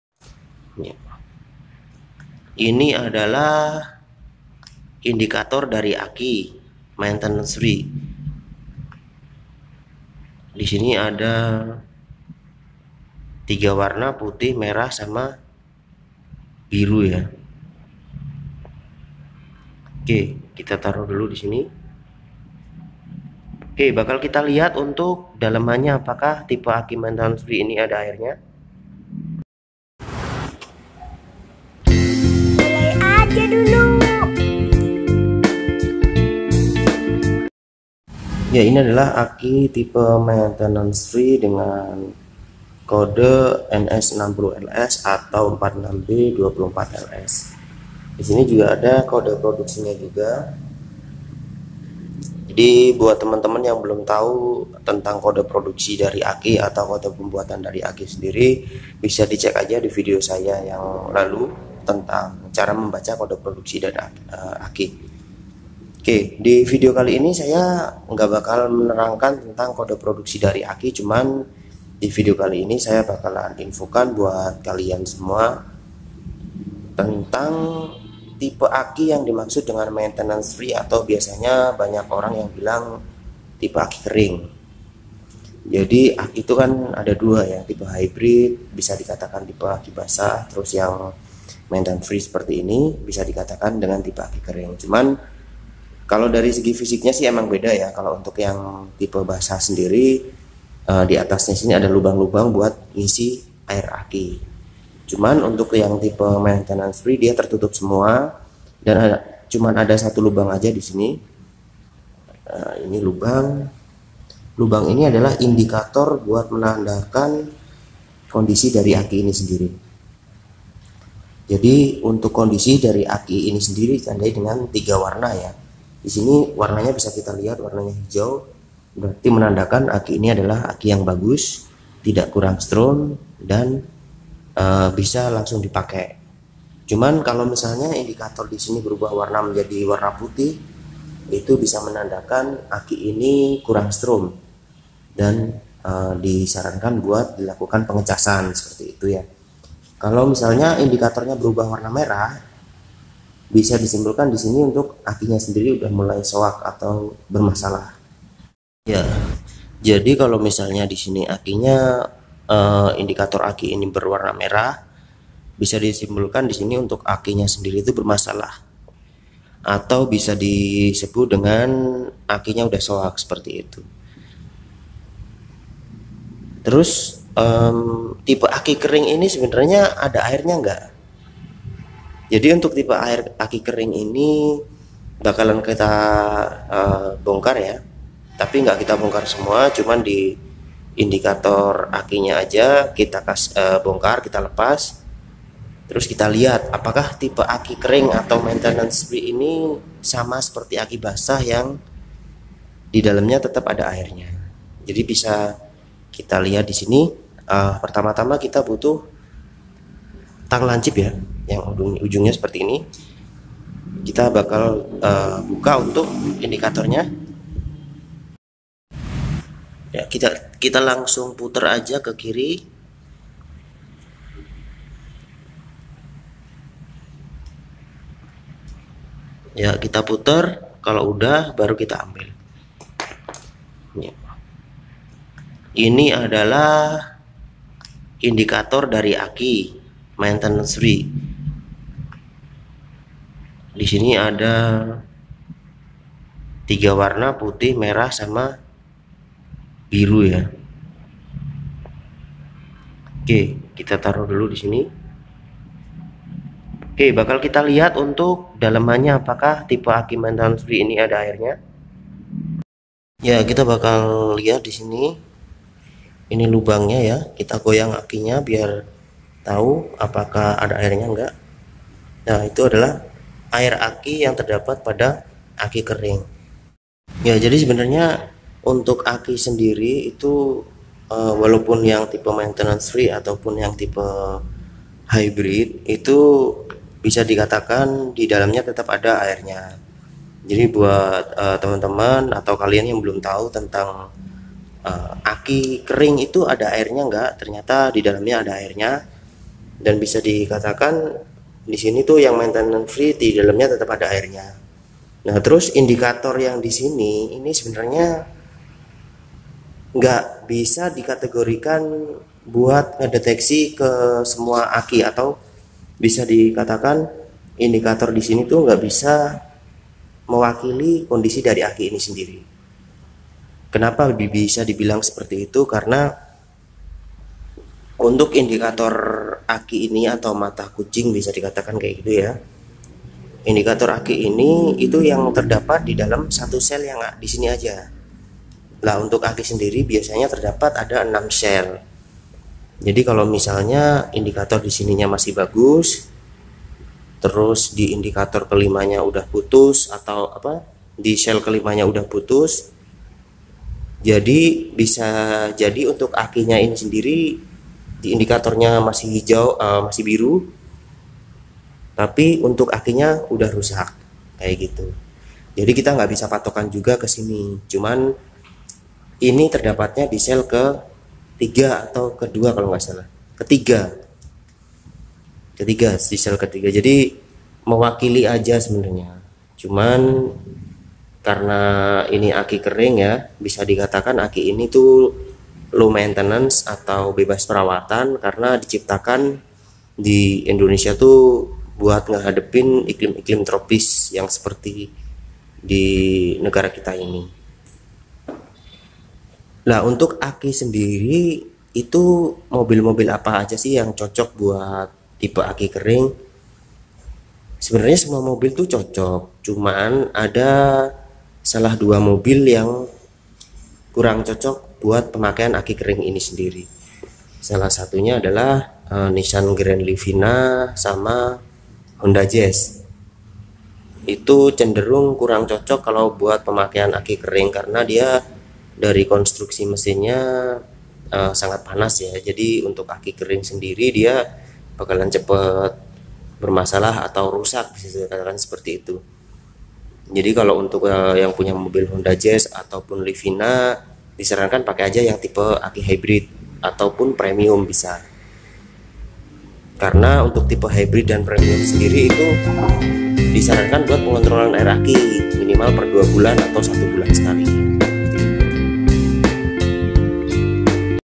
AKI KERING MOBIL || AKI KERING ADA AIR AKINYA
Guys akikering yg kita anggap benar benar kering,ternya adaairnya guys ,cek youtube @studioananta.